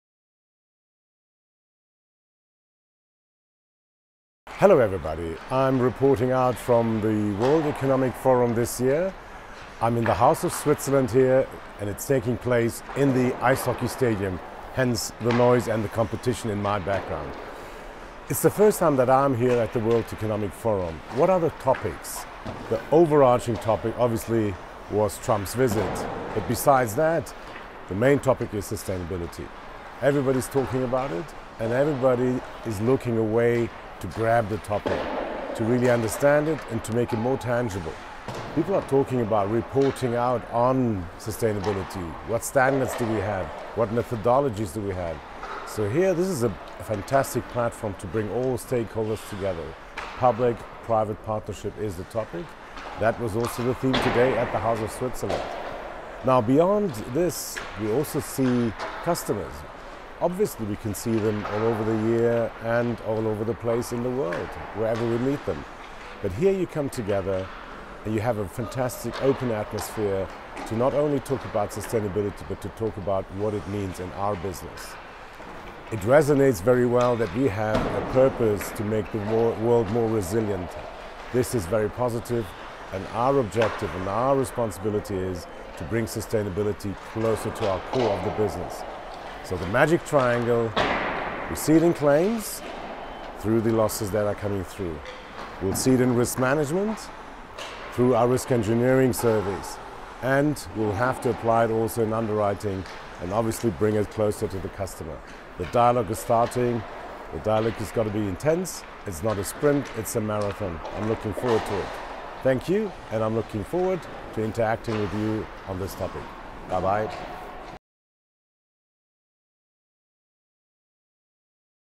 World Economic Forum 2020 – all about sustainability
This year's World Economic Forum was all about sustainability, reflects Andreas Berger, CEO Swiss Re Corporate Solutions. When exploring how we can help companies embrace the risks and opportunities emerging from a focus on sustainability, the WEF is a fantastic platform as it brings all stakeholders together.